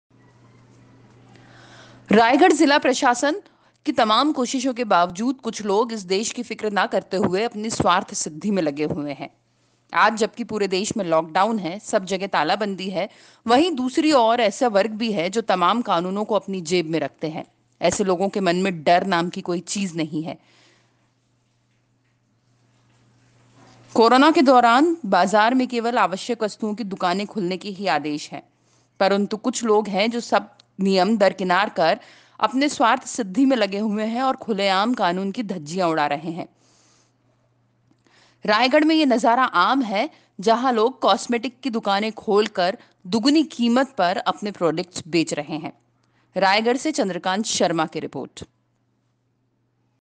लाठी डंडा खा लेंगे दुकान फिर भी खोलेंगे
रायगढ़ जिला प्रशासन पुलिस, प्रशासन, के तमाम कोशिशो के बावजूद कुछ मुद्राराक्षस इस देश की फिक्र ना करते हुए अपने स्वार्थ सिद्धि में लगे हुए हैं .आज जबकि पूरे देश में लाकडाउन है . सब जगह तालाबंदी है .वहीं दूसरी ओर जो धनिक वर्ग है ये लोग तमाम कानूनों को अपनी जेब में रखते हैं . ऐसे लोगो के मन मे डर,नाम की कोई चीज नही रह गई है. एवं बाजार में केवल आवश्यक वस्तुओं की दुकाने खुलने के ही आदेश हैं . परंतु कुछ लोग सब को दरकिनार कर अपने स्वयं के स्वार्थ सिद्धि के लिए सरकार द्वारा बनाए गए कानूनों की खुलेआम धज्जियां उड़ा रहे है . ये लोग अपनी दुकानें खोलकर कॉस्मेटिक सामान  आदि दुगुनी कीमत पर बेहिचक बेच रहे हैं ,रायगढ़ से चंद्रकांत शर्मा की रिपोर्टिंग